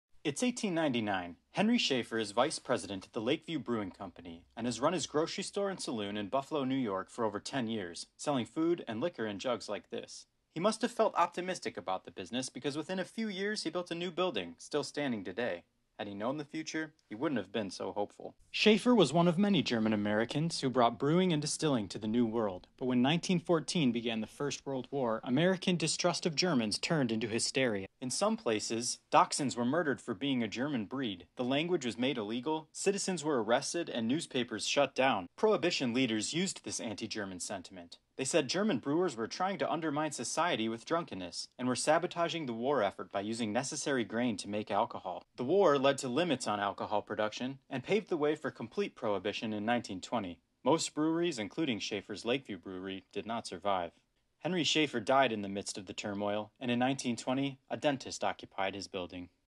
Did the First World War Kill This Business? - H Schaefer Buffalo, NY Whiskey Jug
I bought this stoneware jug in an estate and it opened a lens into the challenges that Buffalo's German population faced during World War 1 and Prohibition.